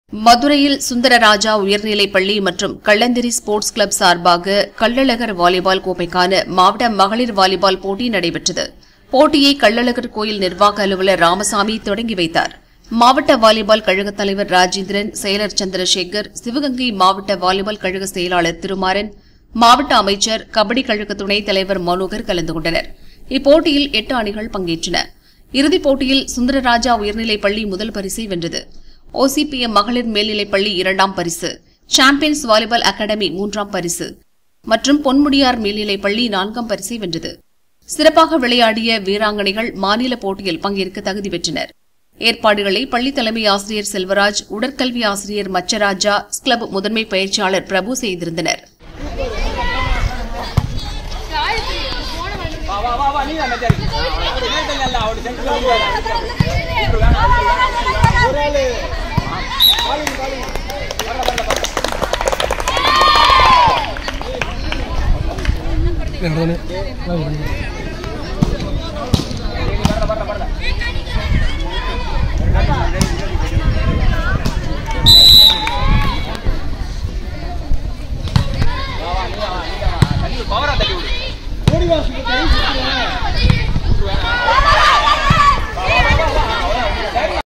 மாவட்ட மகளிர் வாலிபால் போட்டி சுந்தர ராஜா பள்ளிக்கு கோப்பை
மதுரையில் சுந்தர ராஜா உயர்நிலைப் பள்ளி மற்றும் கள்ளந்திரி ஸ்போர்ட்ஸ் கிளப் சார்பாக கள்ளழகர் வாலிபால் கோப்பைக்கான மாவட்ட மகளிர் வாலிபால் போட்டி நடைபெற்றது.

போட்டியை கள்ளழகர் கோயில் நிர்வாக அலுவலர் ராமசாமி தொடங்கி வைத்தார். மாவட்ட வாலிபால் கழகத் தலைவர் ராஜேந்திரன், செயலர் சந்திரசேகர், சிவகங்கை மாவட்ட வாலிபால் கழகச் செயலாளர் திருமாறன், மாவட்ட அமைச்சர் கபடி கழகத் துணைத் தலைவர் மனோகர் கலந்து கொண்டனர். இப்போட்டியில் 8 அணிகள் பங்கேற்றன.

இறுதி போட்டியில் சுந்தர ராஜா உயர்நிலைப்பள்ளி முதல் பரிசை வென்றது. ஓ சி பி எம் மகளிர் மேல்நிலைப்பள்ளி இரண்டாம் பரிசு, சாம்பியன்ஸ் வாலிபால் அகாடமி மூன்றாம் பரிசு மற்றும் பொன்முடியார் மேல்நிலைப்பள்ளி நான்காம் பரிசை வென்றது.

சிறப்பாக விளையாடிய வீராங்கனைகள் மாநில போட்டியில் பங்கேற்க தகுதி பெற்றனர்.

ஏற்பாடுகளை பள்ளி தலைமை ஆசிரியர் செல்வராஜ், உடற்கல்வி ஆசிரியர் மச்ச ராஜா, ஸ்போர்ட்ஸ் கிளப் முதன்மை பயிற்சியாளர் பிரபு செய்திருந்தார்.